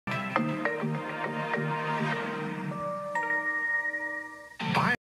Brand New 2022 Dotdash Meredith Logo Endtag! 1/14/2022.
Since Gray Television completed the acquisition of Meredith Corporation since Wednesday December 1, 2021, I decided to check out the brand new Dotdash Meredith logo endtag!